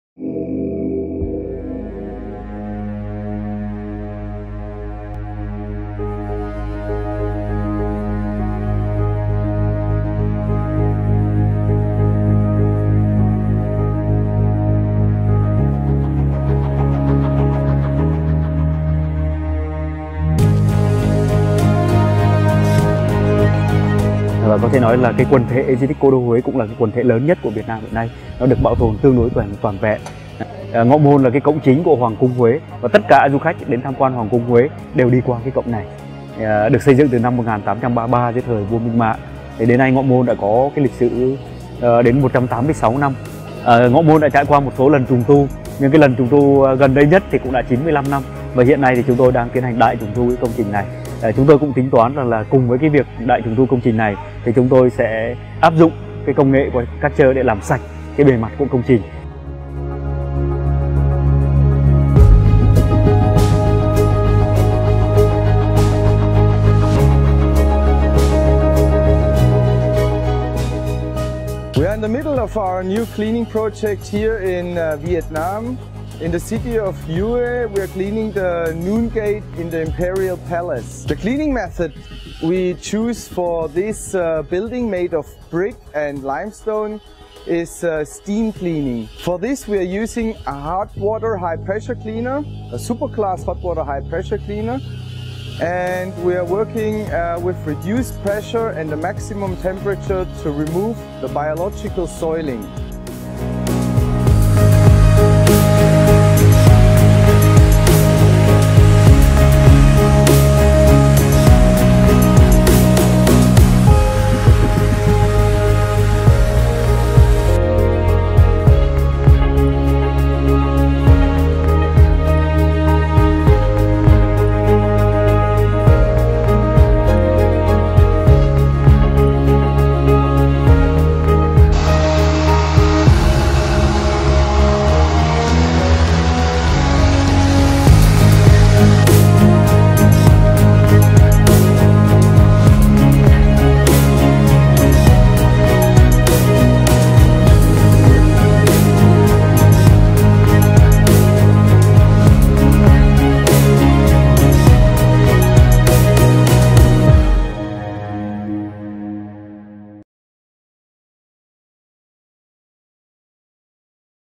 Kärcher Cleaning Project Vietnam Noon Gate Long
As part of our cultural sponsoring, Kärcher cleans the southern main gateway of the former imperial city of Huế in Vietnam. Over decades, the tropical climate, with plenty of rain and high humidity, has led to the original colour of the brickwork and the grey limestone being covered in dark organic vegetation. This damages the façade’s surface in the long term. Kärcher cleaning experts remove these deposits from Noon Gate, which was erected in 1804 and is the largest of the four main gates that frame the imperial city. Huế was built by Emperor Gia Long based on the model of the Forbidden City in Beijing, and it has been a UNESCO World Heritage Site since 1993.